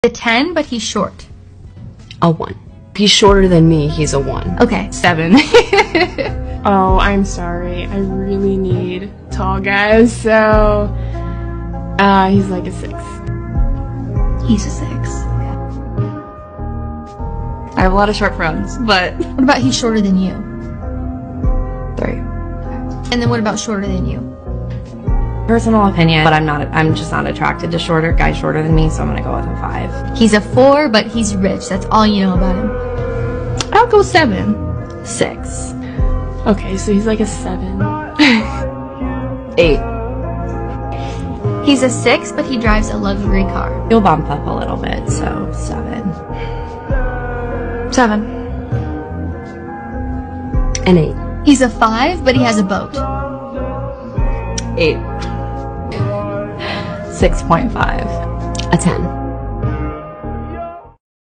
Modern Women Shows Just How A Man’s Attraction Goes Up When He Brings These Things To The Table
😂😂😂😂 All I could do is just laugh while watching this. Just another example of how women will change the narrative when the man has money or materialistic things to offer. That’s why I never listen when women act like these things do matter because it’s clear that they definitely do.